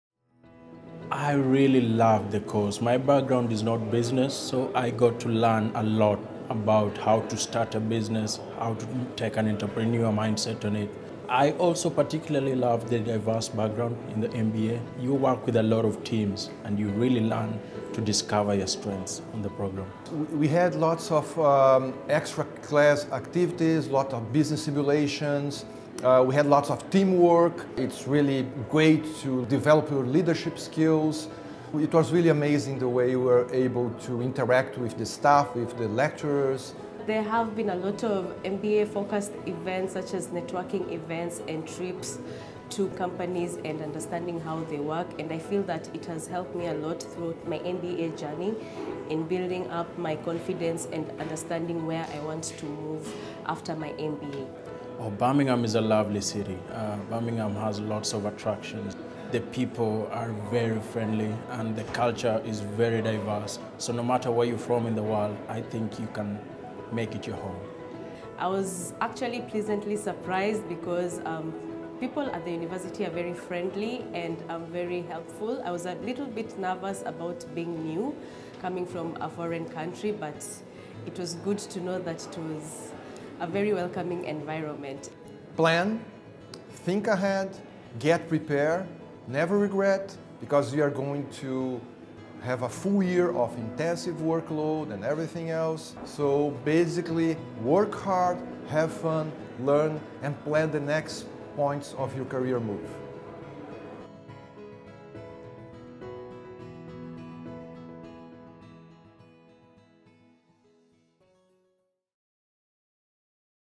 The Birmingham MBA experience
Three MBA students on the cusp of graduating offer some insights into their year at Birmingham.